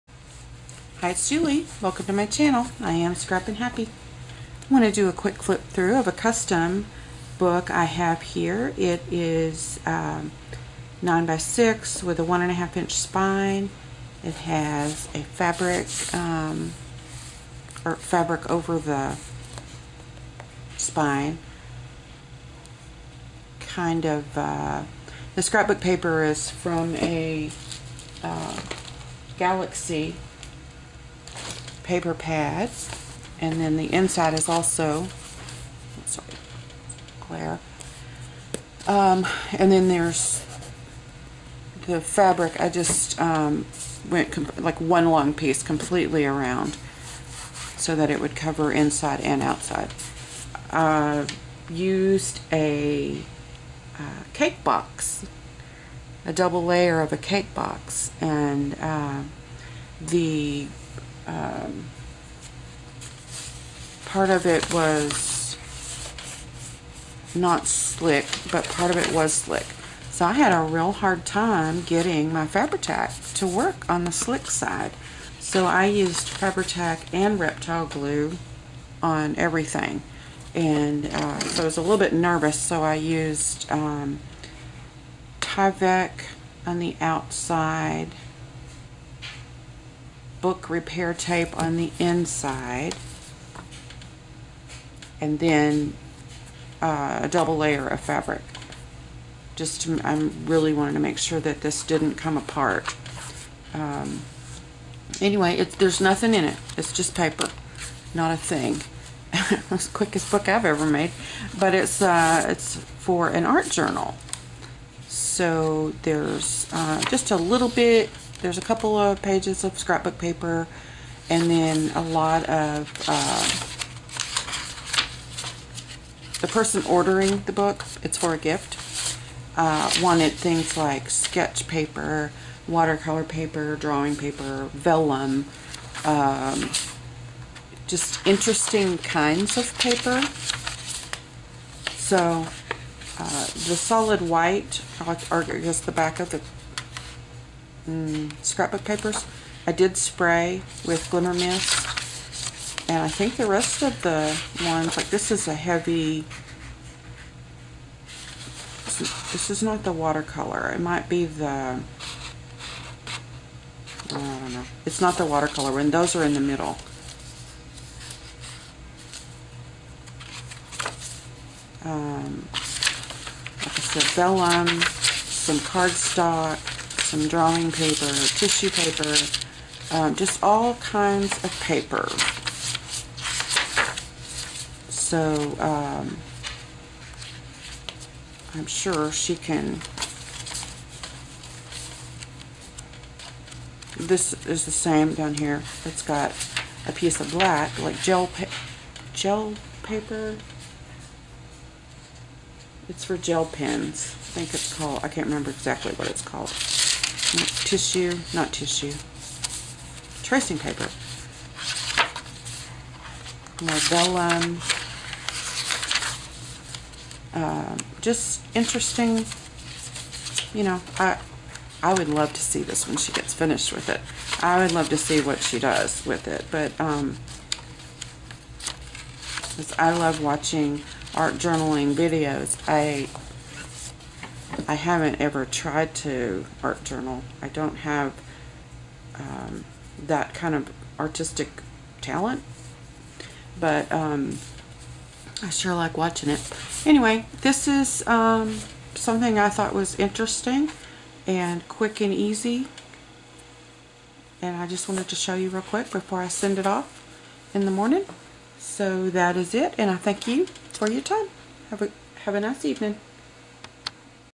Art Journal
Custom Order: 9x6" hard cover blank art journal filled with drawing paper, watercolor paper, vellum, tracing paper, scrapbook paper, and plain paper.  I would LOVE to see this journal when the recipient is finished with it!